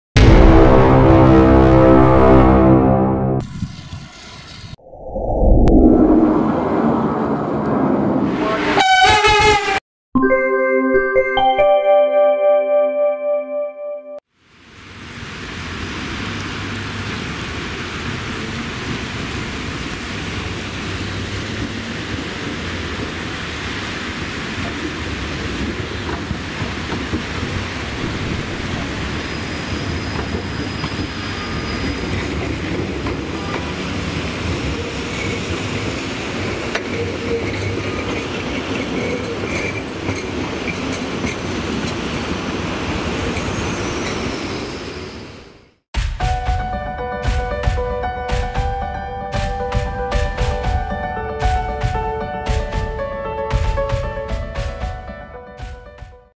Встреча поколений: трамвай "Витязь" (71-931) №0109 и ЛМ-99К (71-134К) №0420 во время ливня
Трамваи "Витязь" (71-931) №0109 и ЛМ-99К (71-134К) №0420 (Приписаны в ГУП "ГорЭлектроТранс",совмещённый трамвайно-троллейбусный парк)
Маршрут №100 -
"Ж.д.ст."Ручьи" - Придорожная аллея" и №58 -
"Ул.Жени Егоровой - Придорожная аллея".

Пр.Просвещения.
Город федерального назначения Санкт-Петербург,Россия.
7.VIII.2021